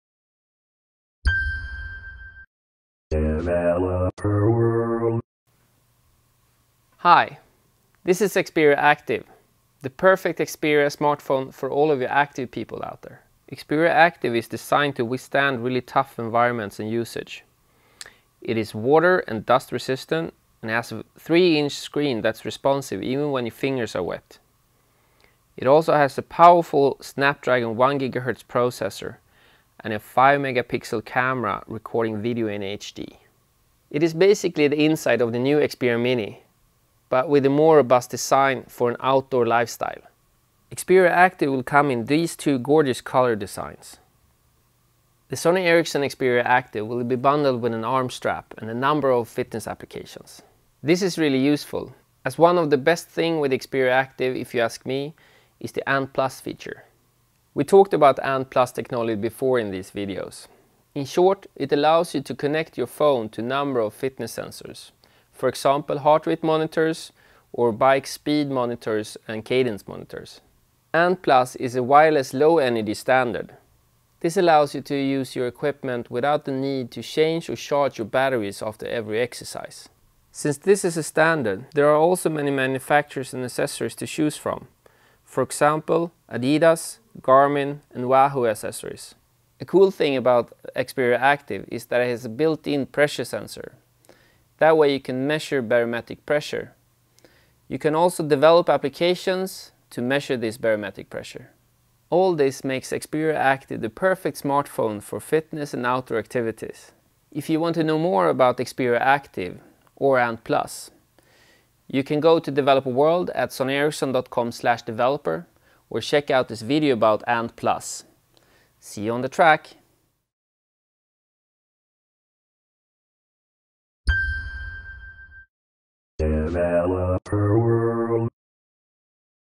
Sony Ericsson Xperia active (feat. a unique pressure sensor and supporting ANT+)
Marcus from Developer World shows the new Xperia active from Sony Ericsson. It supports ANT+ wireless technology for connecting sport and fitness equipment. A truly unique feature of the Xperia™ active lies in its pressure sensor for barometric measuring. Developers can use this sensor in applications, which can be accessed through the Android sensor API. Xperia active is classed as IP67, which means that it can be under 1 meter (3 feet) of water for 30 minutes.